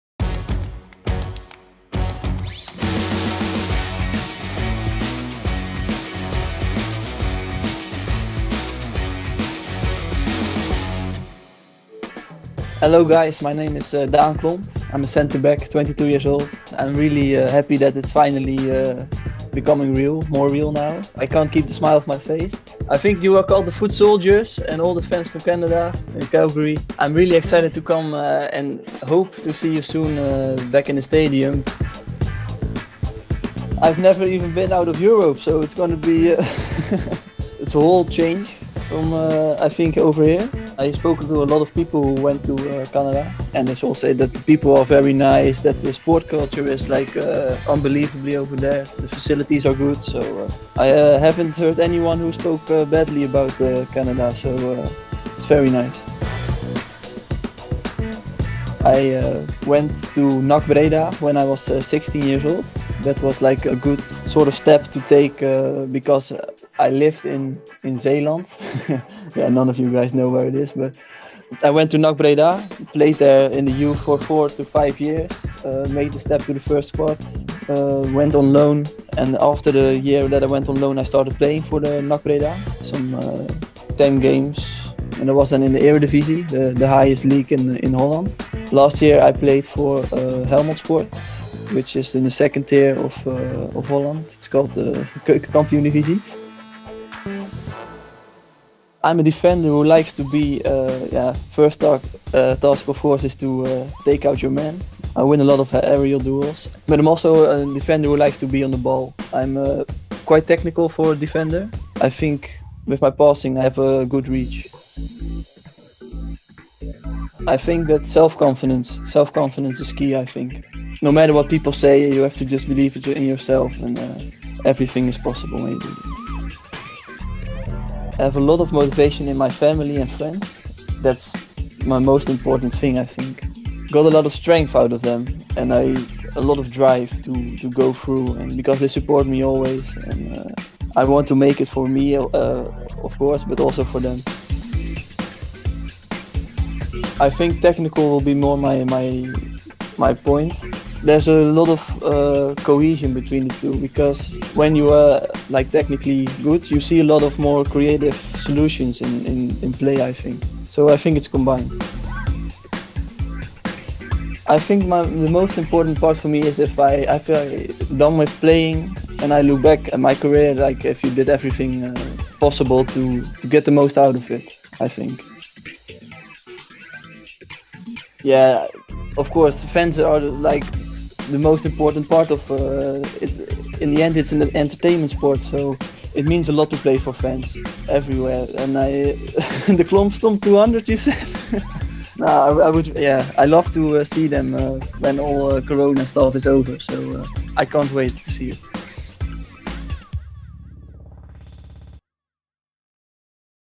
The New Faces of Cavalry FC - Daan Klomp (NED)
The 2021 season of the Canadian Premier League is just around the corner and Cavalry FC  has signed a number of new faces to the roster.

The Cavalry FC Media Team had some around-the-world calls with these new faces to get to know them better.

Dutch Centre-Back Daan Klomp is ready to cross the big pond and get onto the pitch to train. Let's get to know Daan!